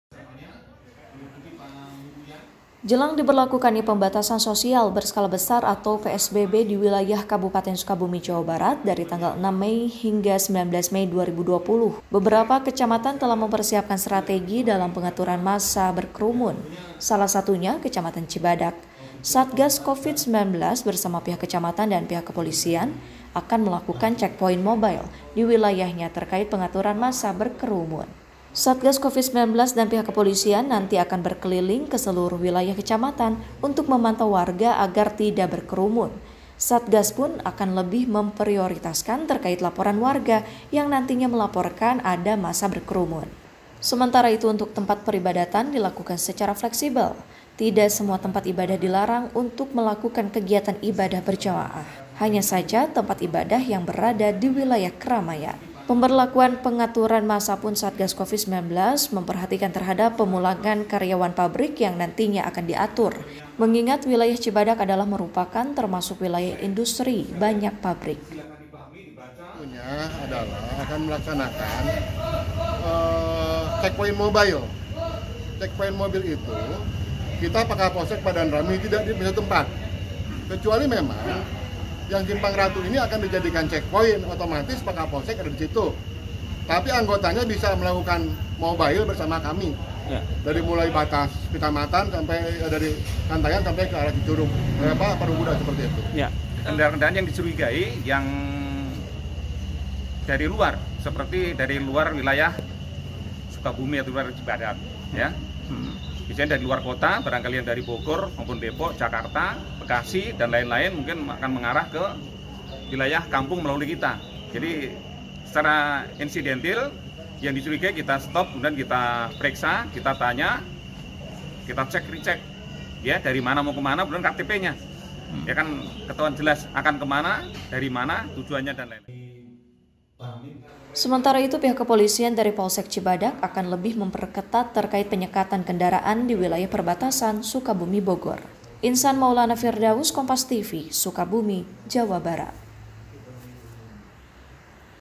Jelang Psbb Sukabumi
SUKABUMI, KOMPAS.TV - Jelang diberlakukannya pembatasan sosial berskala besar, PSBB, diwilayah Kabupaten Sukabumi, Jawa Barat, dari tanggal 6 Mei hingga 19 Mei 2020. Beberapa kecamatan telah mempersiapkan strategi dalam pengaturan massa berkerumun. Salah satunya Kecamatan Cibadak, satgas Covid 19, bersama pihak kepolisian, akan melakukan check point mobile diwilayanya terkait pengaturan massa berkerumun. Satgad Covid 19 dan pihak kepolisian, akan berkeliling keseluruh wilayah kecamatan, untuk memantau warga agar tidak berkerumun. Satgas pun akan lebih memprioritaskan terkait laporan warga
yang nantinya melaporkan massa berkerumun.Sementara itu, untuk tempat peribadatan dilakukan secara fleksible. Tidak semua tempat ibadah dilarang untuk melakulan kegiatan ibadah berjamaah.Hanya saja tempat ibadah yang berada di wilayah keramaian. Pemberlakuan pengaturan massa pun satgas Covid 19, memperhatikan terhadap pemulangan karyawan pabrik yang nantinya akan diatur. Karena wilayah Cibadak adalah termasuk wilayah industri yang banyak pabrik. Sementara itu pihak kepolisian dari Polsek Cibadak, akan lebih memperketat terkait penyekatan kendaraan diwilayah perbatasan Sukabumi-Bogor.